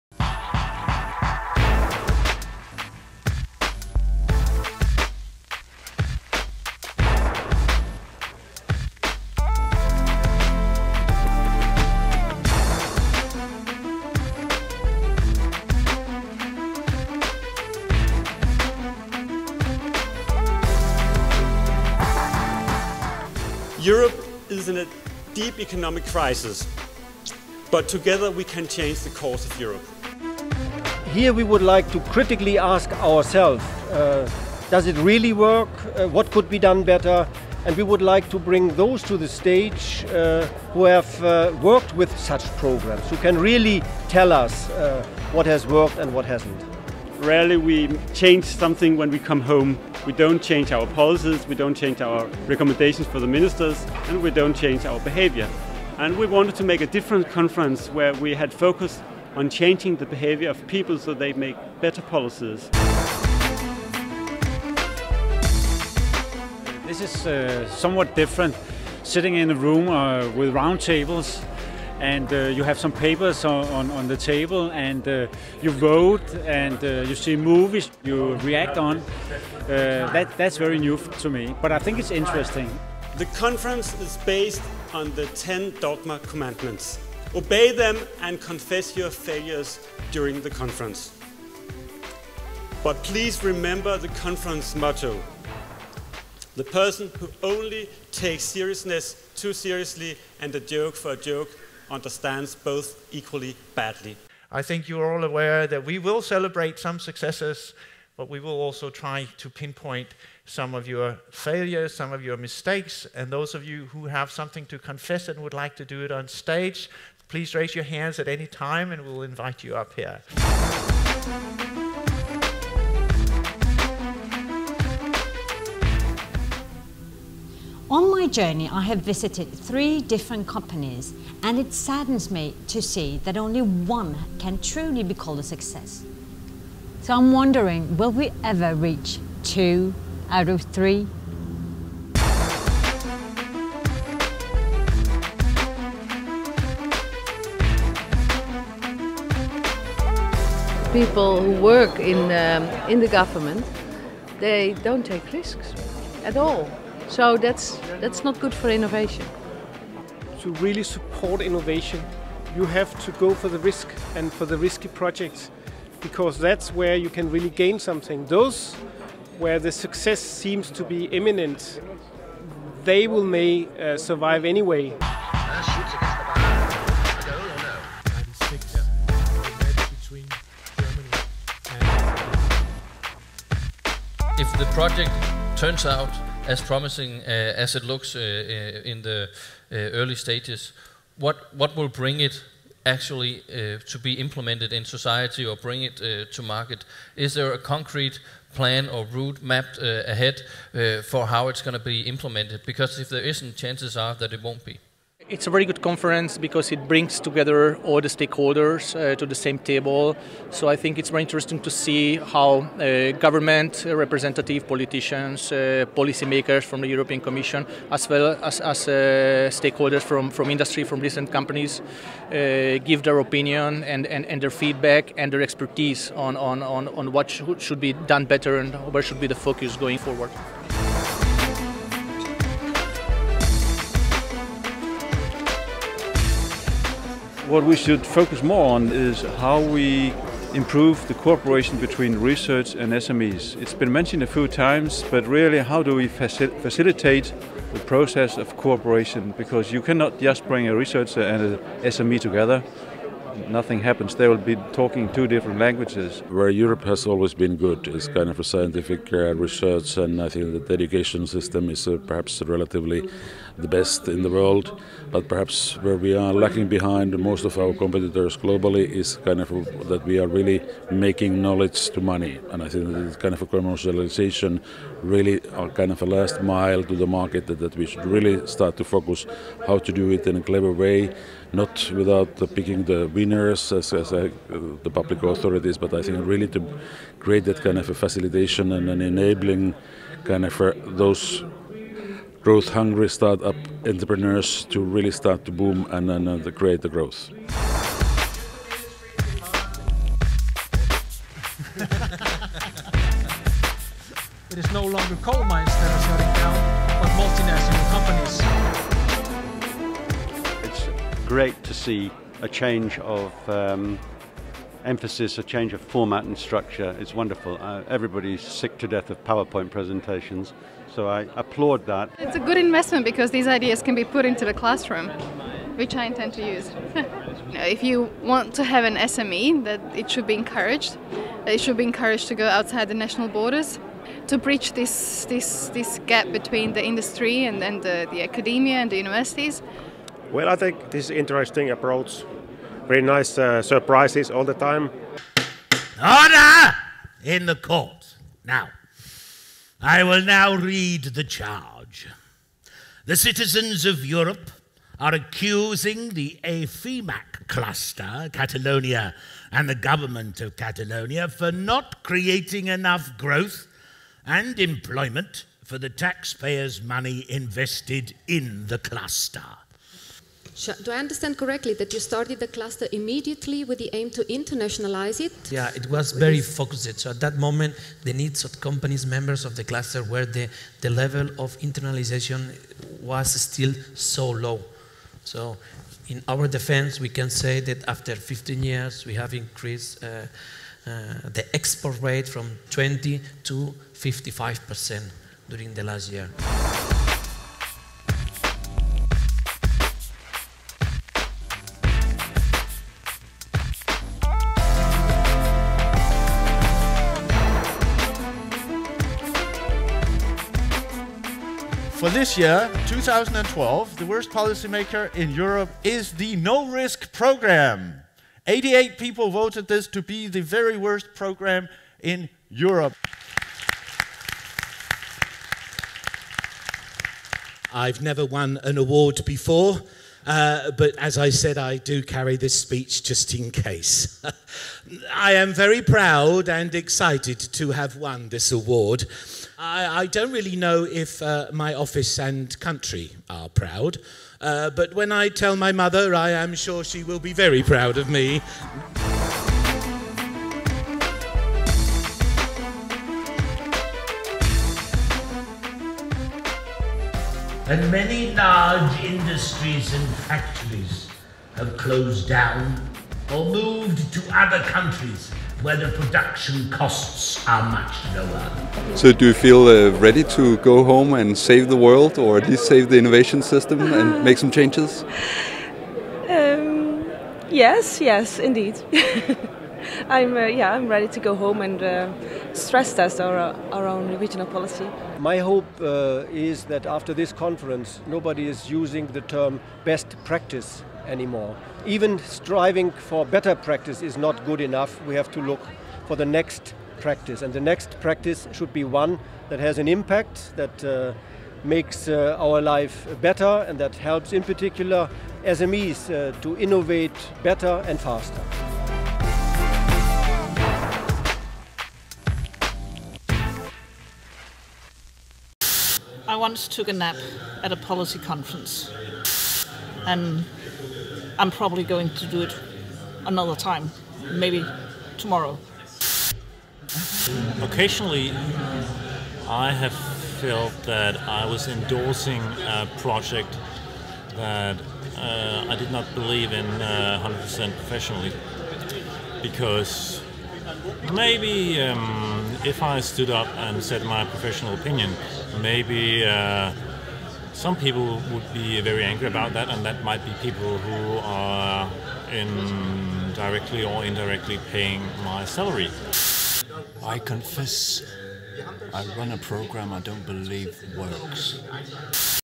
Europe INNOVA 2012 Conference in Copenhagen
The Europe INNOVA 2012 Conference, held in Copenhagen on October 23-24, was not your typical EU conference. The approach to European innovation support was unusual, slightly provocative and straight to the point. The conference featured not only the usual success stories, but also less successful policies and projects were given a central position at the event. Watch a short summary of the conference here.